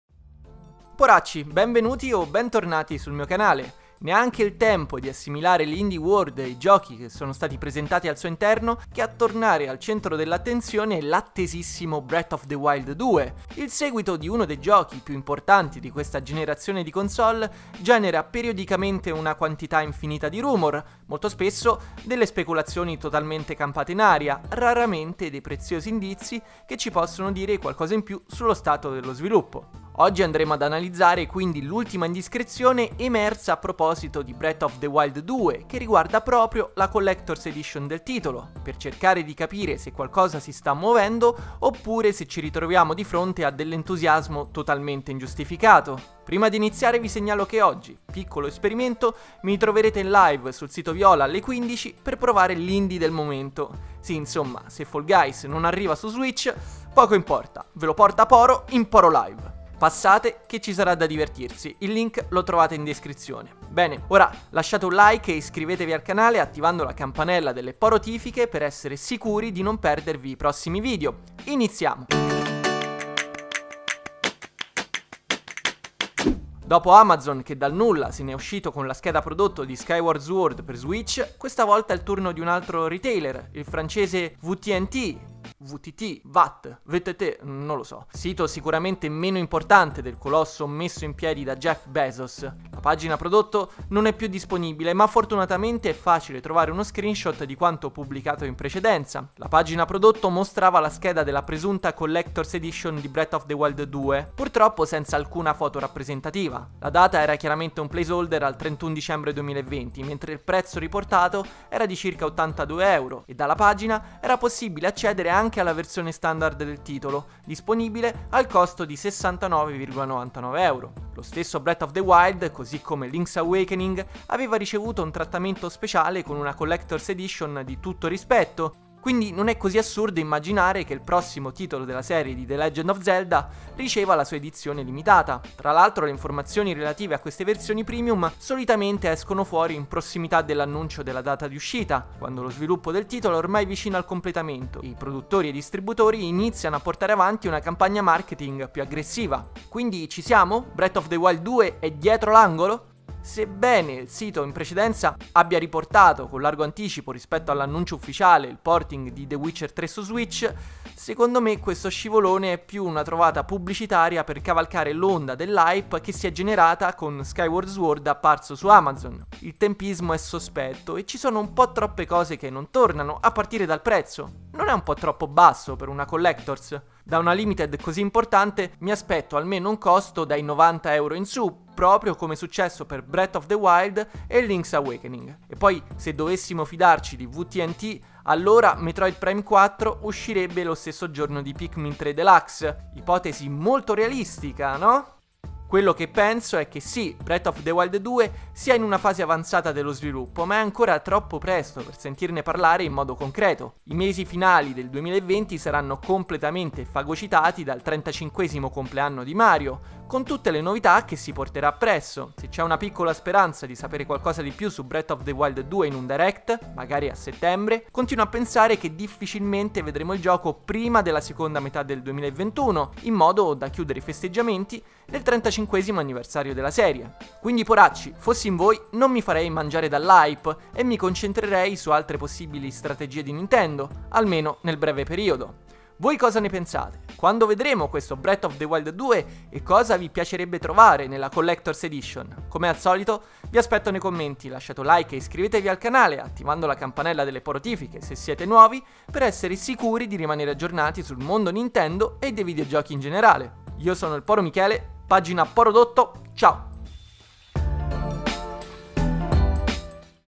Svelata la Collector's Edition di Breath of the Wild 2? Facciamo CHIAREZZA [Rumor - Leak ITA]
Continuano i rumor e le indiscrezioni su The Legend of Zelda. breath of the Wild 2. Il seguito di uno dei giochi più importanti di questa generazione di console genera sempre molto hype e i fan sono impazienti di scoprire di più sul titolo, a partire dalla data di uscita. Su un sito francese è apparsa la pagina relativa a quella che sembrarebbe essere la collector's edition di Breath of the Wild 2. Che Nintendo abbia intenzione di annunciare a breve la data di botw 2 e della sua limited edition? Ci sarà un'altra Mster Sword? Fatemi sapere cosa ne pensate nei commenti!

Io sono il Poro Michele, PORODOTTO, ciao!